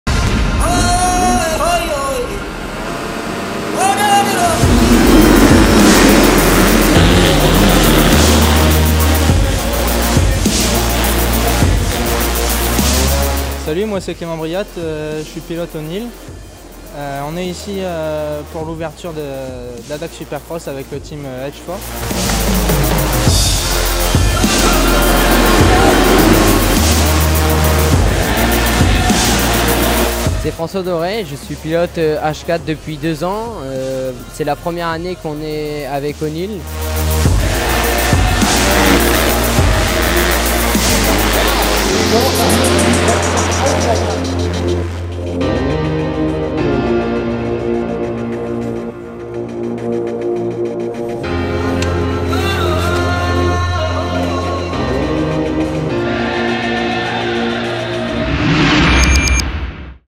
O'Neal introducing the HFour Racing Team
We caught-up with Clément Briatte and Francois Doré of the O'Neal supported HFour Racing Team by Energybody based in Switzerland!

The boys are geared up with O'Neal Gear Kits for the ADAC SX Cup 2018/2019 Series over in Germany!  🔥💪🏼

––– QUICKLINKS –––